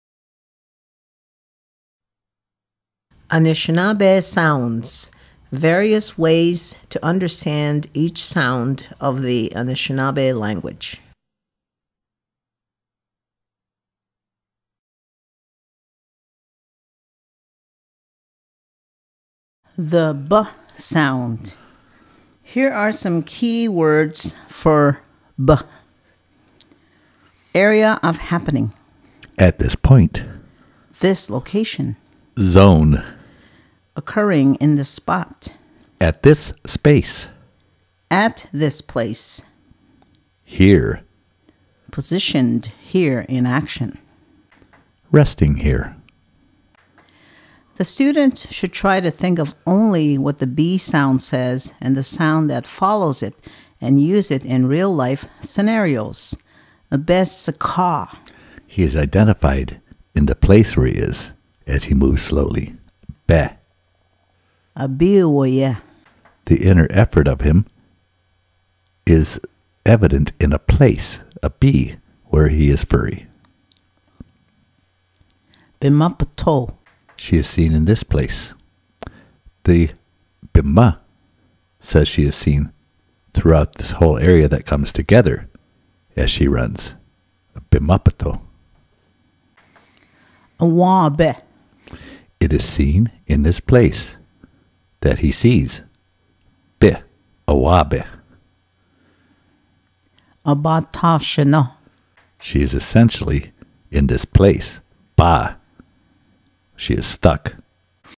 Anishinaabe Sounds Flashcards/DVD Set b sample
This flashcard set comes with videos on each consonant and vowel.  Example words were chosen to cover the concepts spoken about in every consonant-vowel combination.  This is a tool to use in training students to think the true meaning of Anishinaabemowin instead of thinking in feeble English translation/definitions.  Use this with The Sound Based Method of Understanding Anishinaabemowin.  Available for purchase at helenroy.net